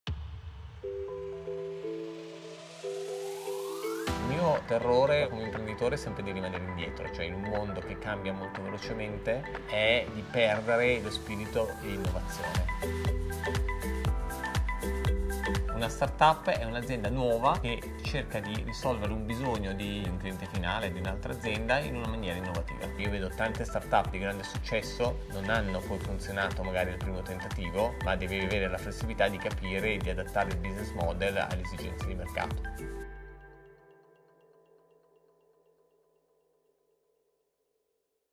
B Heroes 1 | Scopriamo B Heroes | Fabio Cannavale, ci spiega il significato di startup
In poche parole Fabio Cannavale, ideatore del progetto B Heroes, ci spiega che cosa significa essere una startup.

B Heroes nasce da un'idea di Fabio Cannavale e vuole favorire la crescita del tessuto imprenditoriale italiano, sostenendo startup nelle prime fasi di sviluppo e promuovendo una cultura d’impresa attenta alla sostenibilità ed all’impatto sociale.

Seguici anche su tutti i social!